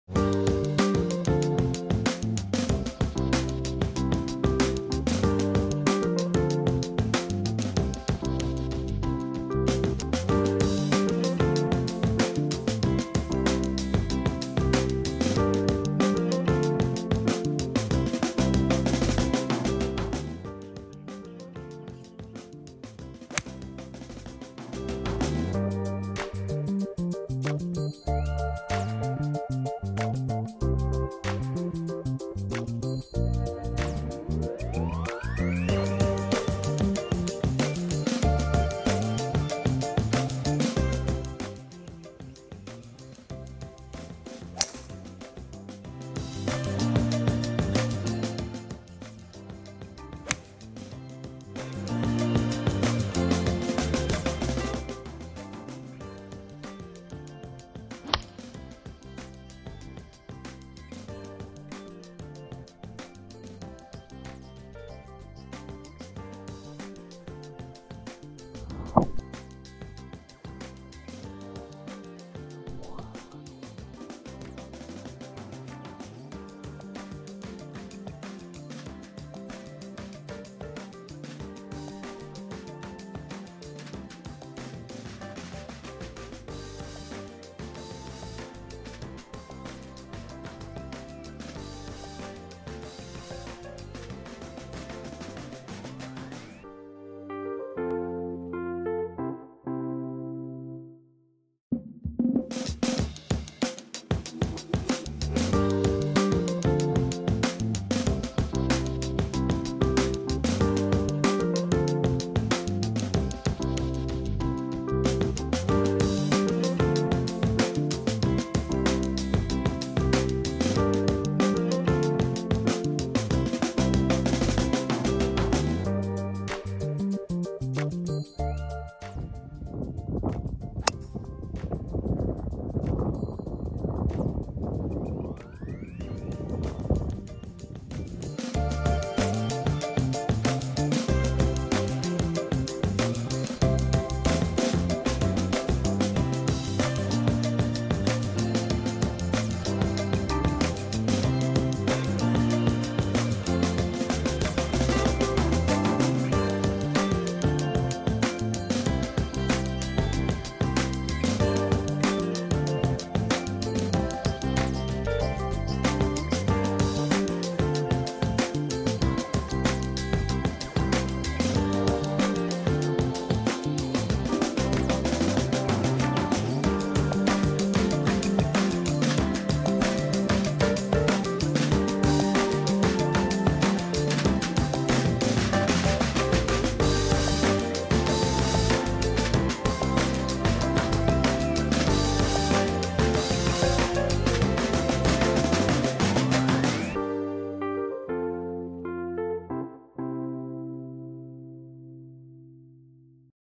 Trying To Fix My Slice
Trying to fix my slice with the driver, as it’s been getting worse and worse.